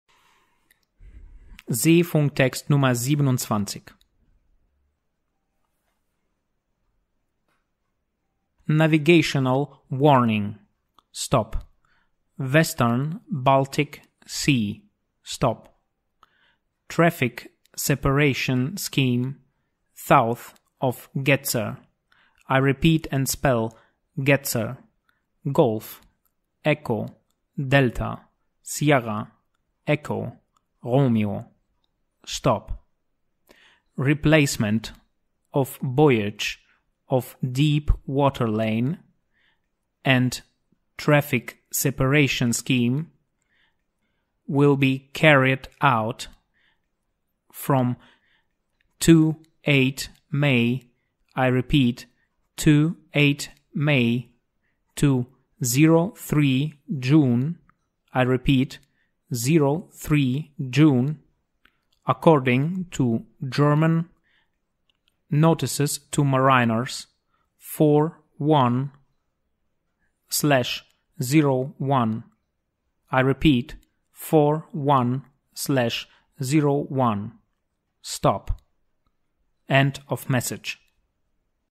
SRC und LRC Seefunktext Nr. 27 - Theorie Prüfung, SegelnAG
Die gesamte Theorie, Tipps und Gerätetraining in unserem SRC Onlinekurs.

Seefunktexte für die SRC- und LRC-Prüfungen zum Übersetzen und fürs Diktat. Eigennamen werden hierbei wiederholt und buchstabiert, Zahlen werden wiederholt und in einzelnen Ziffern durchgegeben.

Englischer Text:
Navigational warning. Western Baltic (Sea): Traffic separation scheme south of Gedser. Replacement of buoyage of deep water lane and traffic separation scheme will be carried out from 28 May to 03 June according to German notices to mariners 41/01.

Deutscher Text:
Nautische Warnung. Westliche Ostsee. Verkehrstrennungsgebiet südlich Gedser. Austausch der Betonnung des Tieffahrwasserlinie und des Verkehrstrennungsgebietes wird laut Nachrichten für Seefahrer 41/01 vom 28. Mai bis 03. Juni durchgeführt werden..